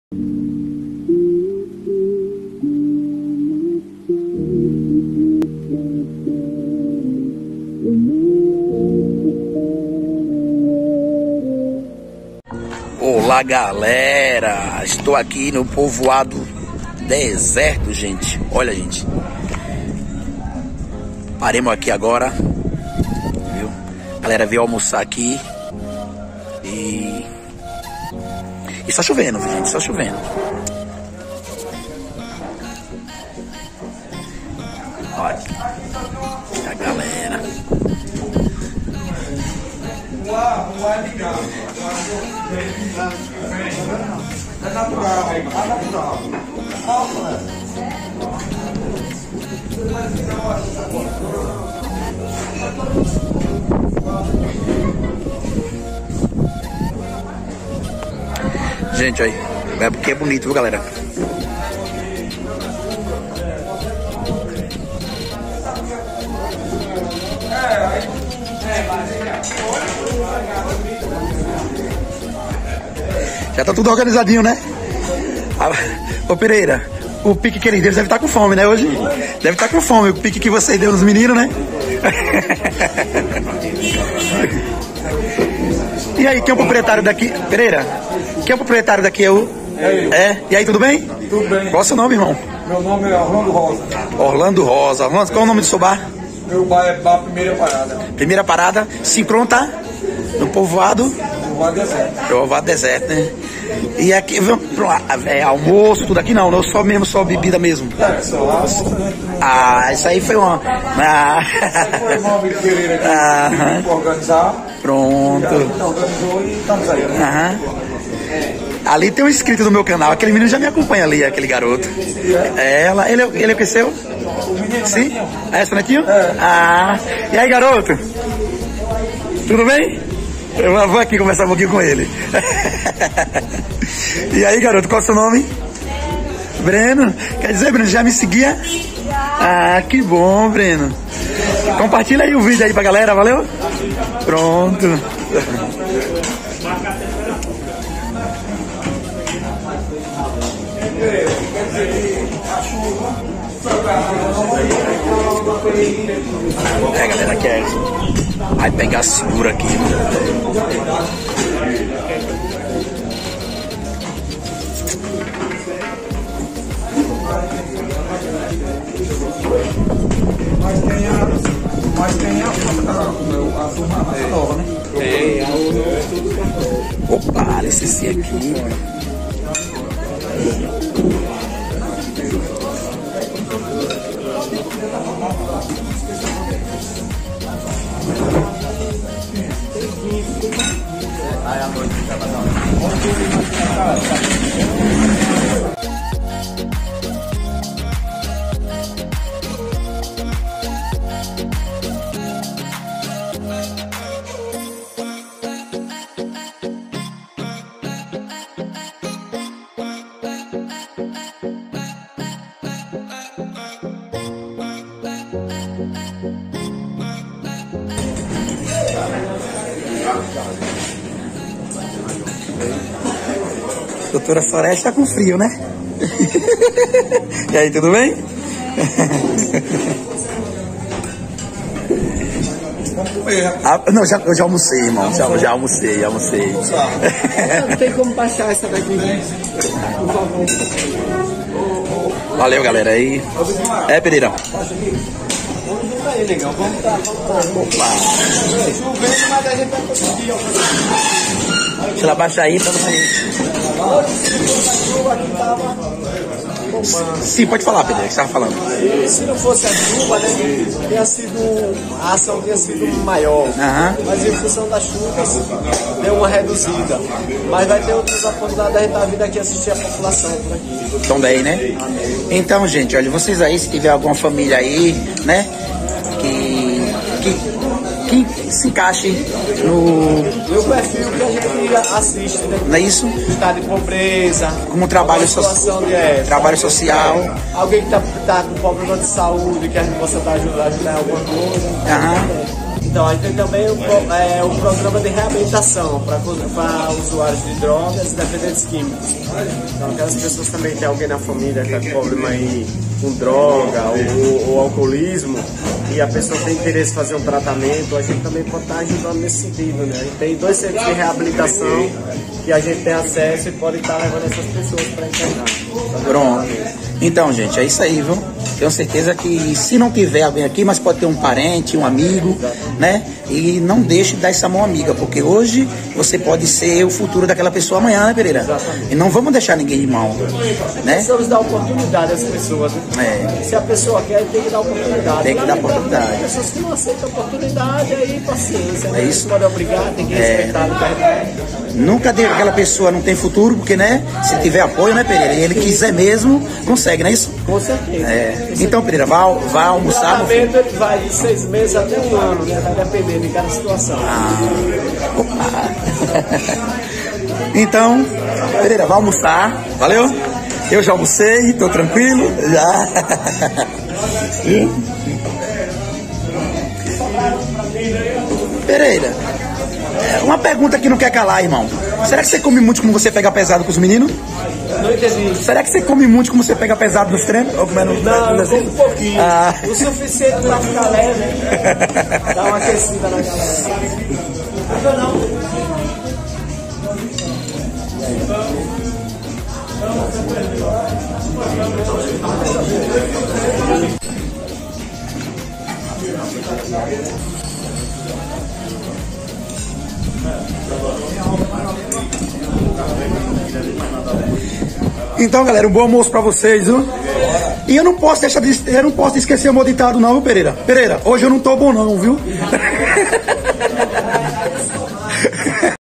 A EQUIPE PAROU PARA ALMOÇAR E DAR CONTINUIDADE AO TRABALHO SOCIAL "BAR 1° PARADA POVOADO DESERTO"
MEU INSTAGRAM

PIX PARA DOAÇÕES FALE PARA QUEM VOCÊS ESTÁ DOANDO QUERO SER O MAIS TRANSPARENTE POSSÍVEL! ↓

OBRIGADO!!..